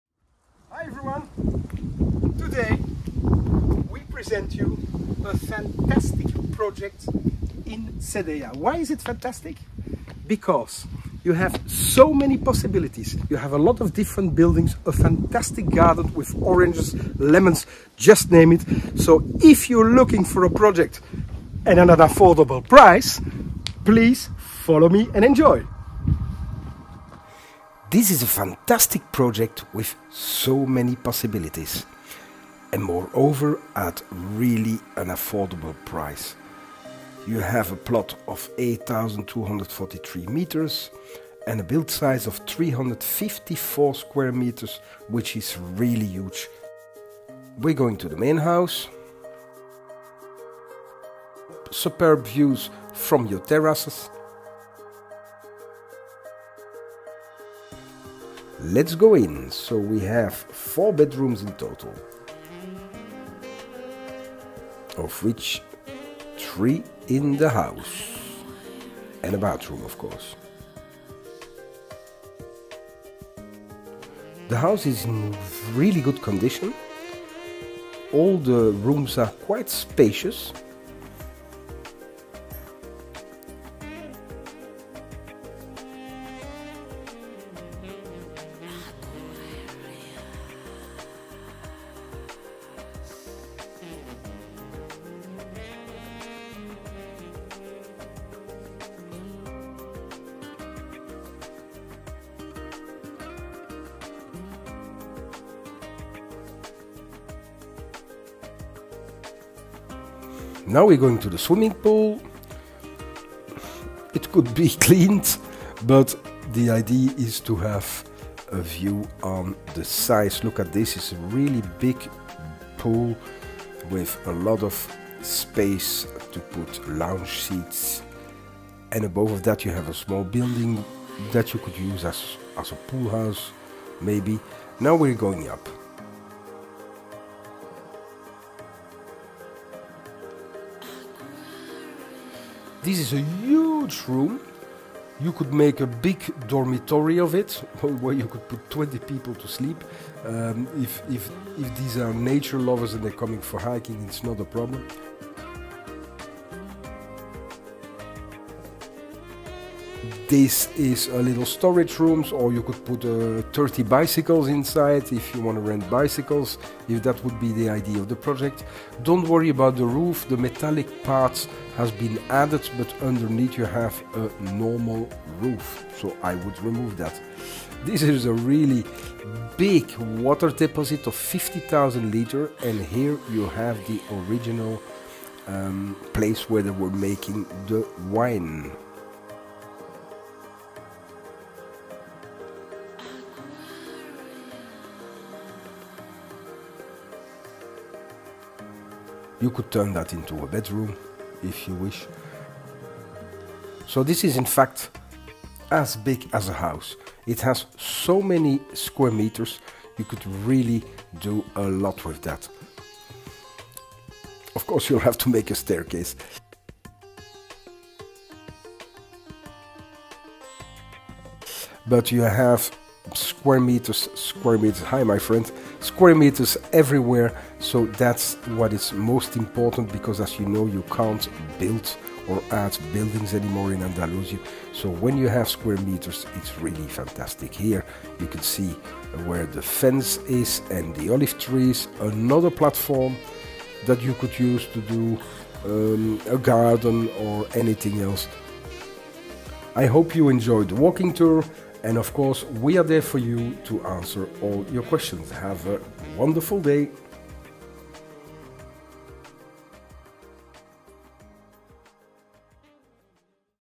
AbraCasaBra walks with you in Cortijo Jose in Sedella - Malaga
This is what we call a project! Endless possibilities in the middle of georgous nature!!
Have you seen the movie and the pictures? Here they are:
Live your dreams! We'll help realising them!!